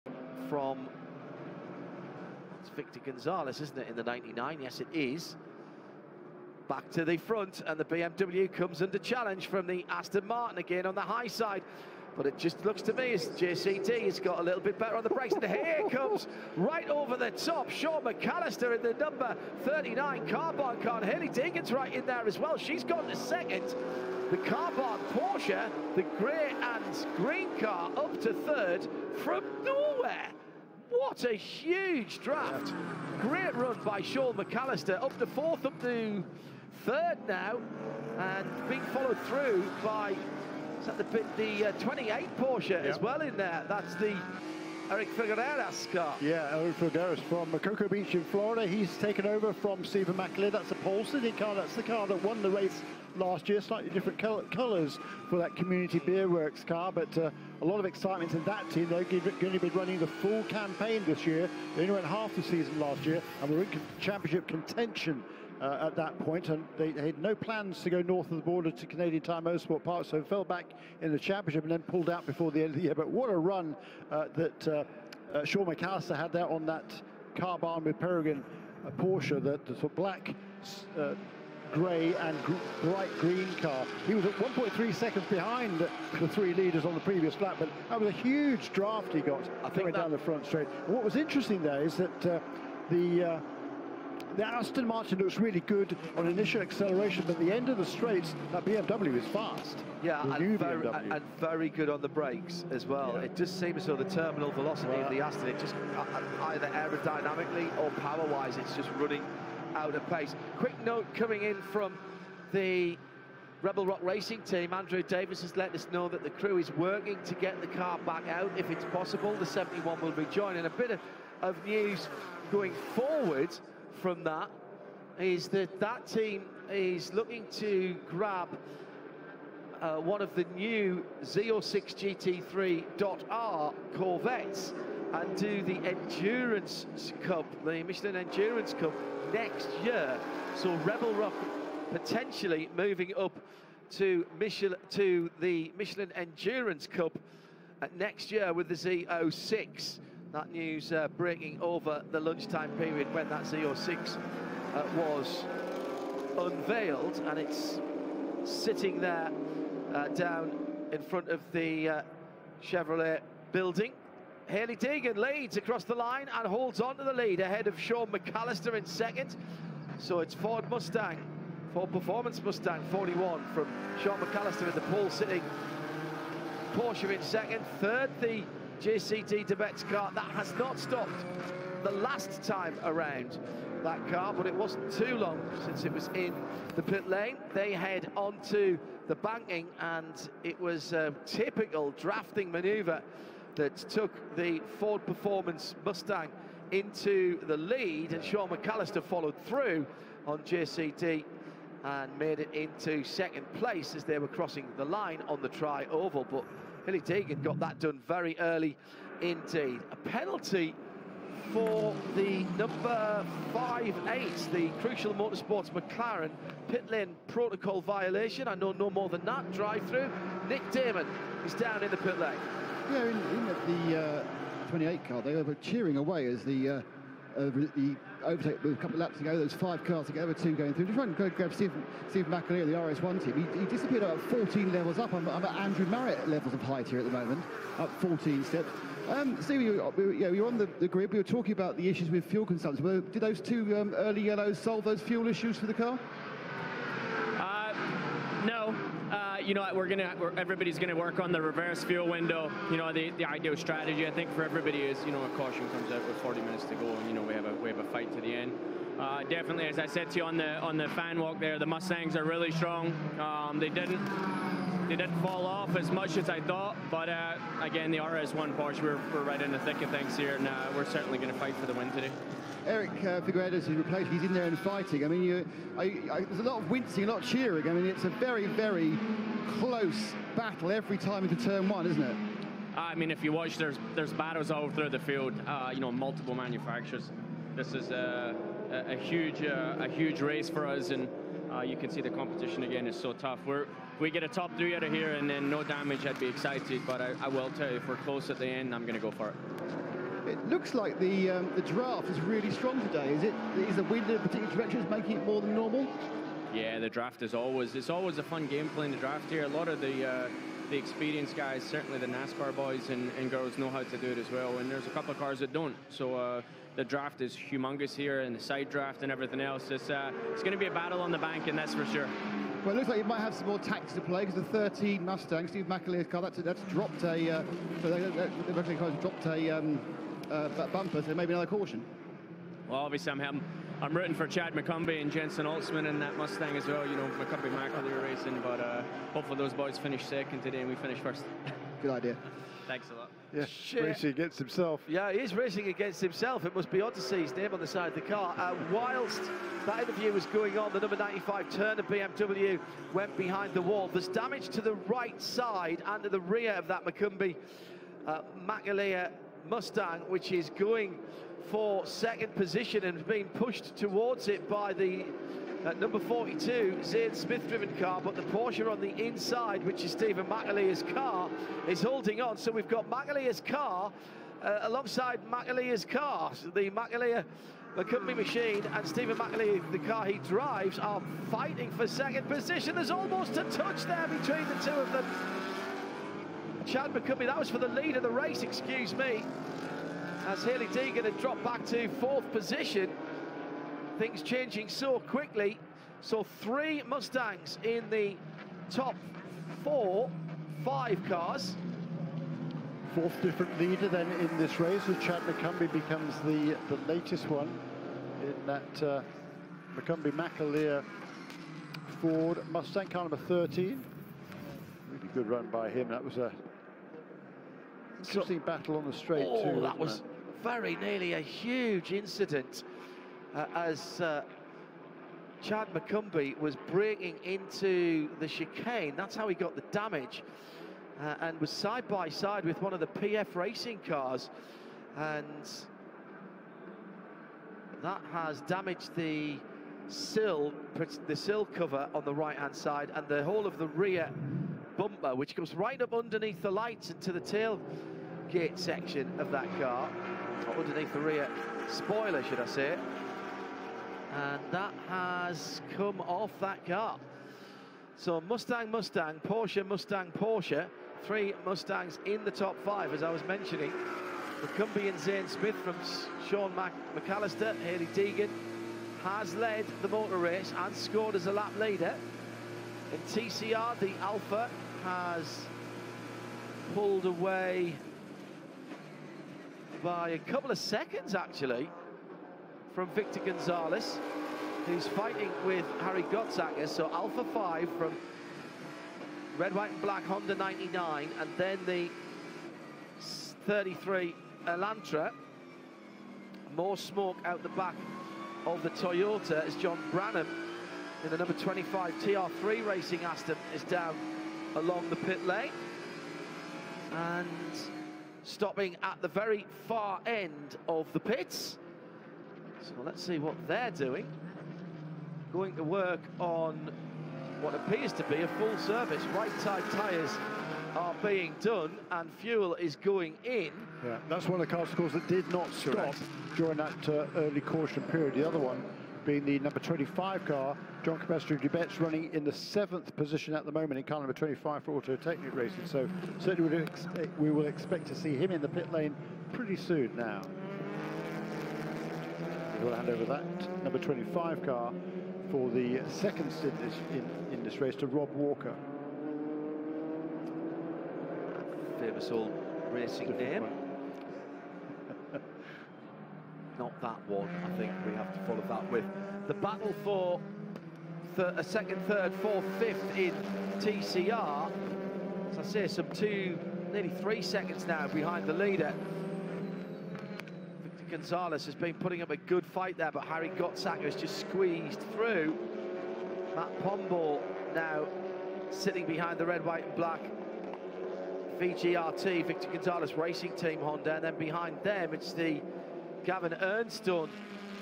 Great Battle for the Lead - 2023 Michelin Pilot Challenge Daytona
Brilliant GT4 battle for the lead from Daytona